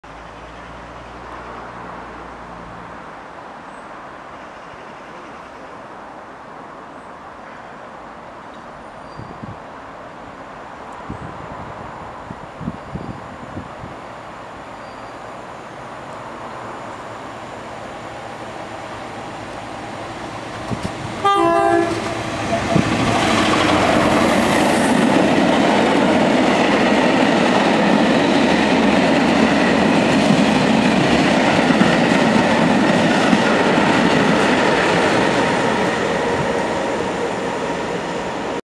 Colas 70802 on 6E07 Boston to Washwood Heath driver training
Water Orton on the 1st August 2014.